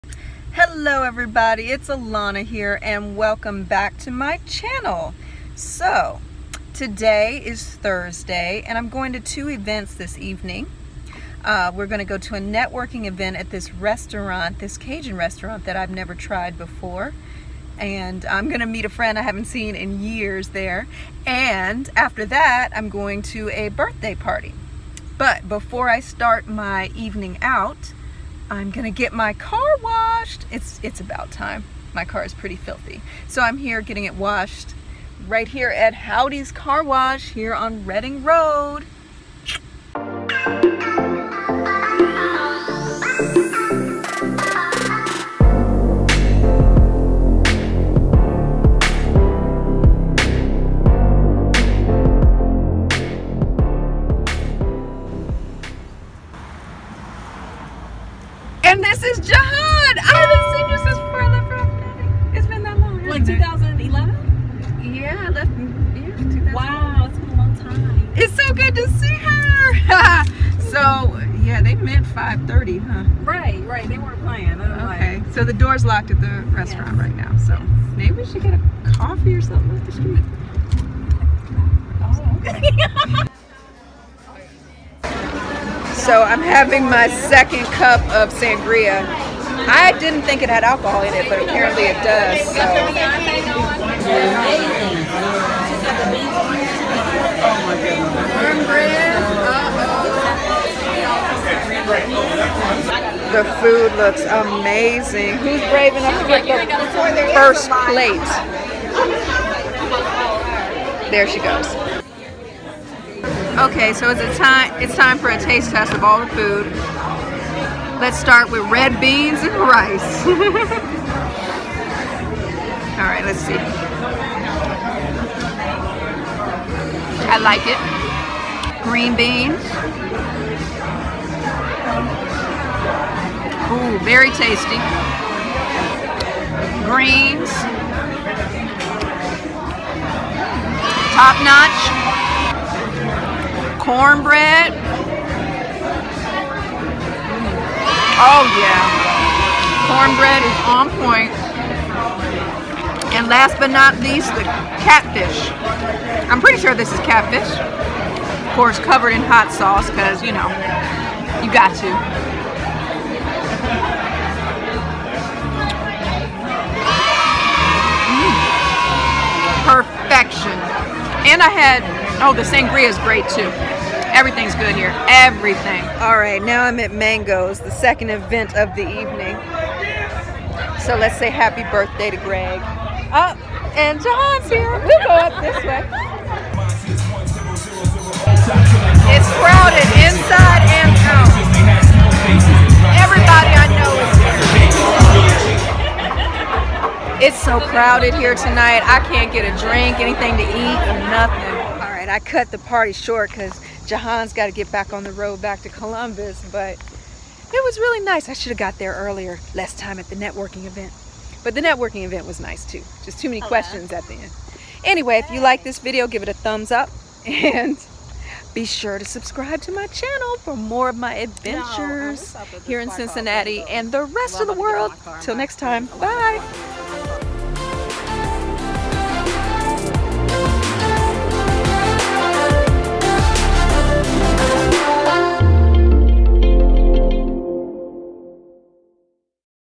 Sweet Home Cincinnati Season 2 Ep. 4 Two Events One Night
Come with me to a networking event at Mardi Gras restaurant where the food was the BEST in town. Then I go to Mango's for a birthday party.

Shot with Canon G7x mark ii